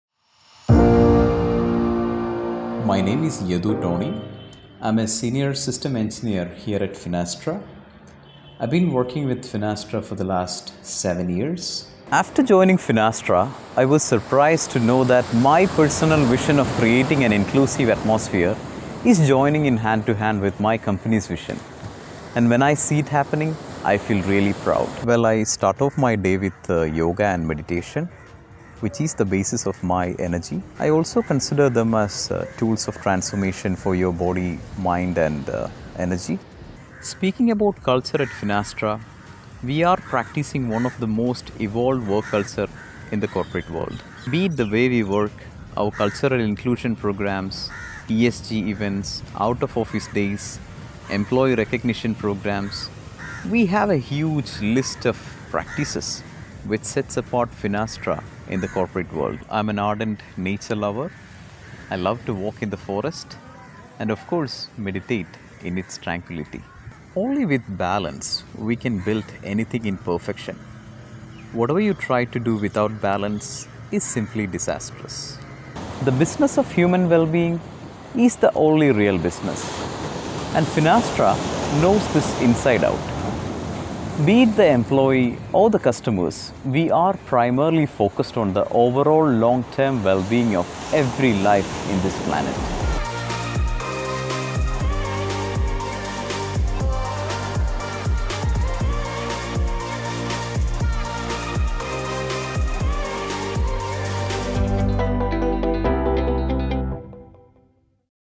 Life at Finastra: Yadhu Tony
Finastra global company and mental health and balance are important to us all year round globally. At Finastra we empower our people to prioritise their own health and well-being so they can be their best selves at home and at work.

At Finastra, we are committed to promoting open conversations about mental health and combating the associated stigmas. We recognize that self-care and balance are vital, which is exemplified by many of our people, like Yadhu Tony, who regularly practices self-care and balance in his daily routine.

We understand that life presents various obstacles, and overcoming them requires the right resources. That's why we provide our employees with unlimited access to specialized coaches who support all aspects of wellbeing.

Together, we are working towards normalizing discussions around mental health and fostering a supportive environment.

By prioritizing self-care and providing the necessary tools, we can thrive both personally and professionally.